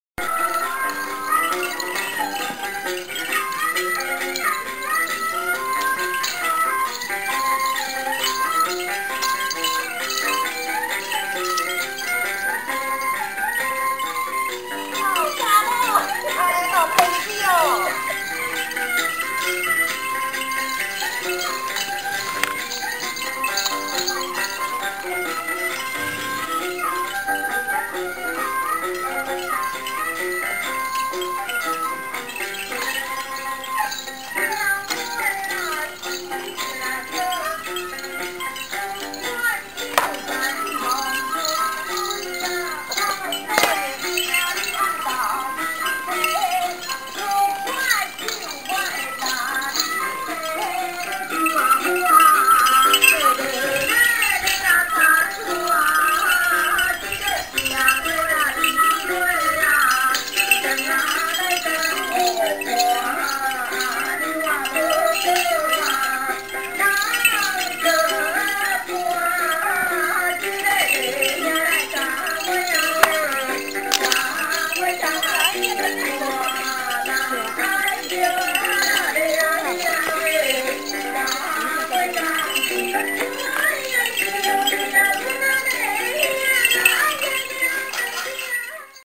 台西鄉光華村牛犁陣表演
台西鄉光華村發展地方民間藝陣不遺餘力，以牛犁陣來展現他們的特色，並且四處表演廣獲好評，保留了傳統的民間技藝。
牛梨陣演出的場地不拘，可定點表演，亦可邊走邊演，上演時三生三旦，且歌且舞相互答唱，有時相互捉弄、調情，牛也是要角之一，穿梭在歌舞中，作拖犁耕田狀。最為逗趣的是那對公婆，不時製造一些打渾的笑科，帶動了整個場面的高潮。
往昔的農村，大都是日出而作，日落而息，生活單調娛樂又少，村人就組成各種設備較為簡單的小戲來打發農閒時間。
早期它是莊嫁人用來自娛，漸漸地在迎神會上也扮演著一個重要的腳色，後來有錢人家遇到喜慶場合也聘請牛犁陣表演，以增日熱鬧氣氛，全盛時期在任何婚喪喜慶場合都可看到牛梨陣的演出。